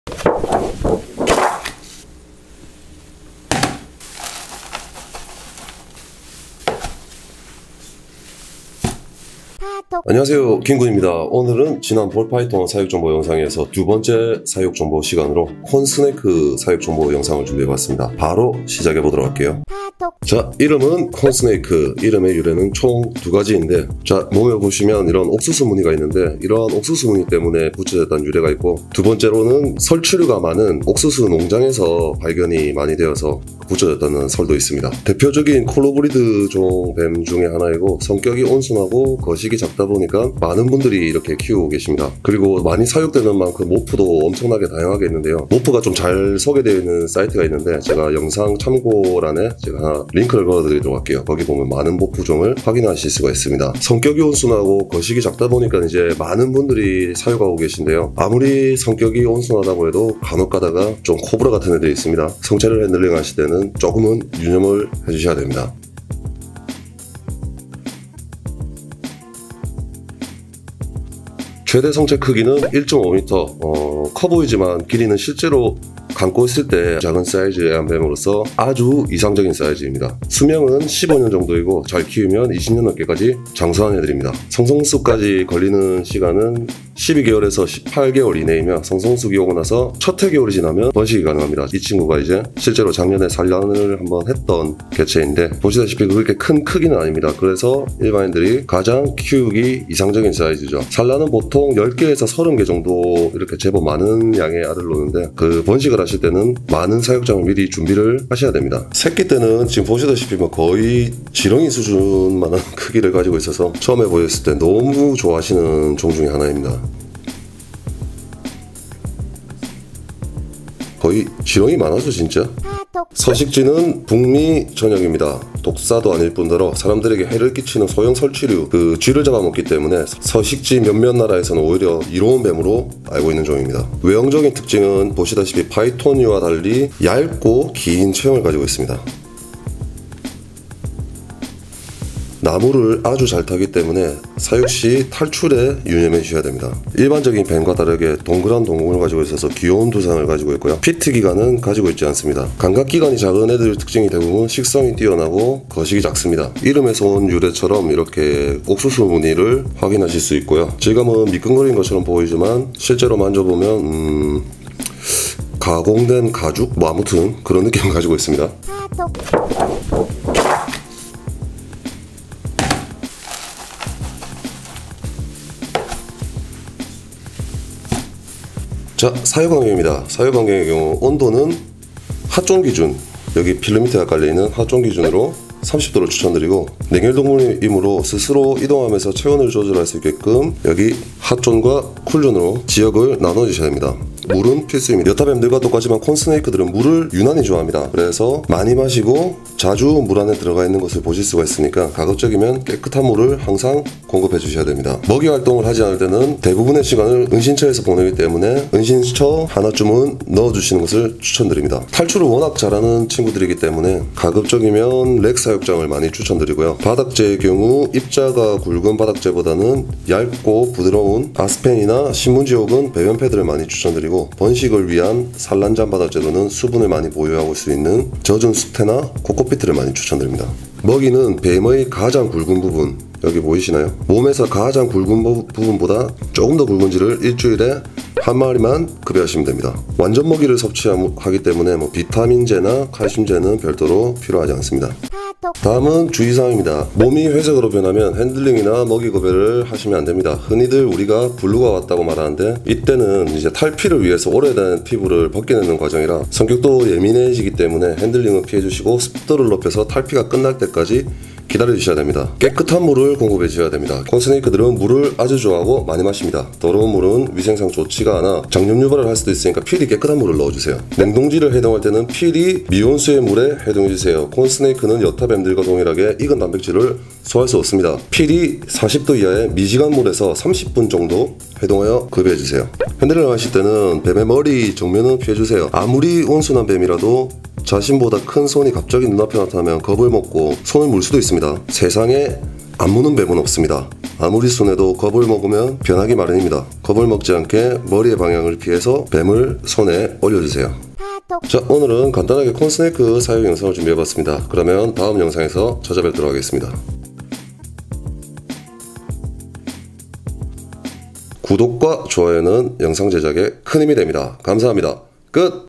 귀여운 뱀! 가장 대중적인 파충류중 하나인 애완뱀 콘스네이크 사육정보
가장 대중적이고 입문자들이 많이 키우는 콘스네이크 사육정보 영상을 준비해봤습니다. 입문자 분들께 많은 도움이 되셨으면 좋겠네요 :)

파충류톡 김군의 정보